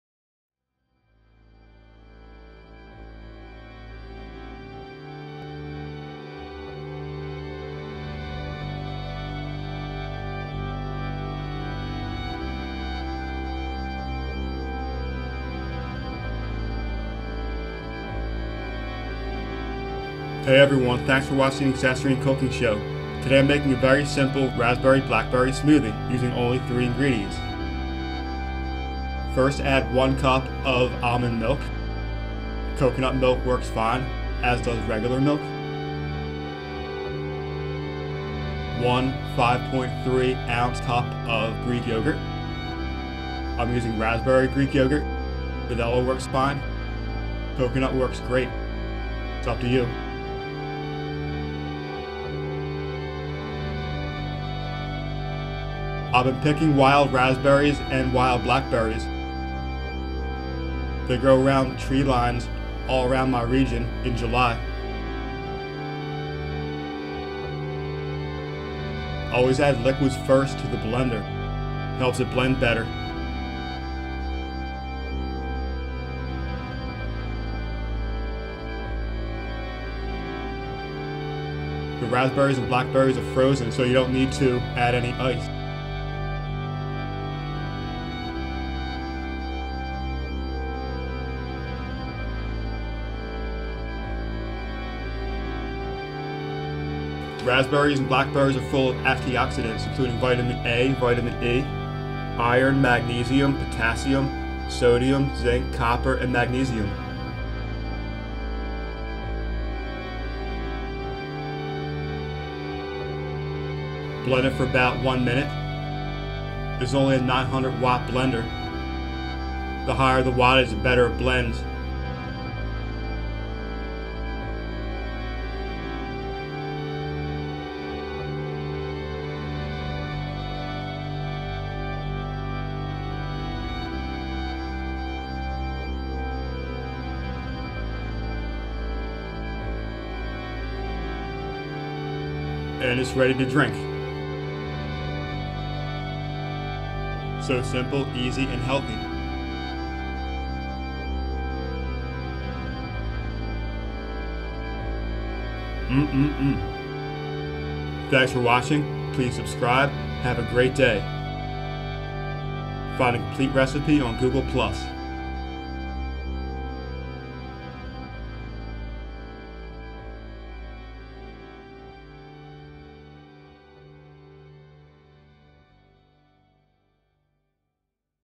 Wild Raspberry and Blackberry Smoothie
This is probably the simplest smoothie recipe there is. It only requires 3 ingredients. Berries freeze well. Just rinse off, put in freezer bags, and freeze.